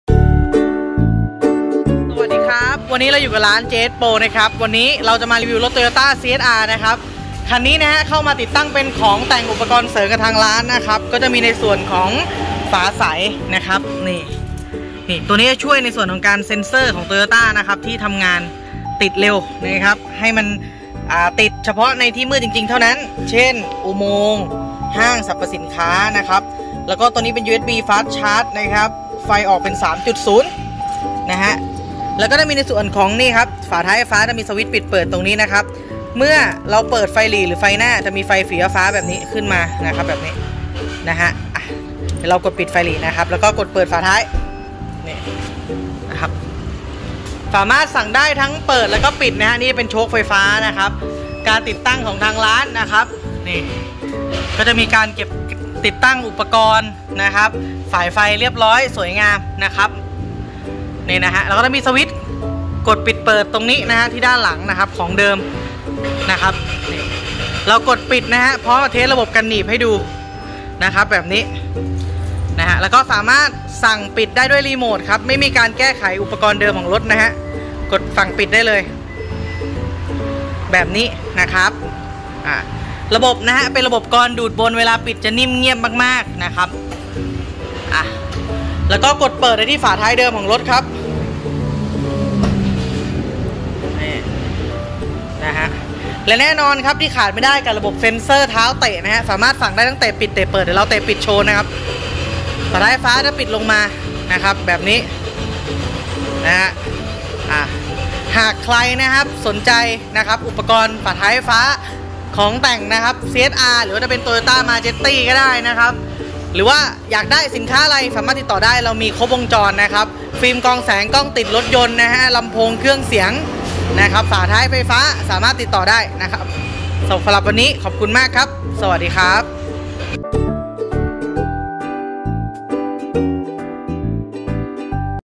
งานติดตั้งฝาท้าย TOYOTA CH-R พร้อมระบบเซ็นเซอร์เท้าเตะ เพียง 12,900.- ฟรี ! ค่าติดตั้ง | by JSPRO
สำหรับคันนี้ลูกค้าเพิ่ม
- USB fast charge
- ฝาใสครอบเซ็นเซอร์
**การันตีผลงานการติดตั้งด้วยช่างมืออาชีพด้านฝาท้ายไฟฟ้าที่ติดตั้งมากกว่า 1,000 คัน!!
💯การันตีรับประกัน 1 ปีเต็ม ❤️
✅กลอนดูดบนปิดเบา/เงียบ
✅ระบบกันหนีบ
✅ชุดสายไฟแบบ Y ปลั๊กไม่มีตัดต่อ
✅สวิตช์หน้าตรงรุ่น
✅เบ้ามือจับหลังเปลี่ยนใหม่ตรงรุ่น
✅มีสวิตช์กดปิดฝาท้ายในตัว
✅สั่งปิด-เปิดฝาท้ายไฟฟ้าด้วยเซ็นเซอร์เท้า
✅ไม่มีการเจาะใดๆที่ตัวถังรถสายไฟเข้ารูยางเดิมของตัวรถ
✅มีระบบเปิดจากรีโมทเดิมรถ ไม่ต้องดัดแปลง

***สนใจนัดคิวติดตั้ง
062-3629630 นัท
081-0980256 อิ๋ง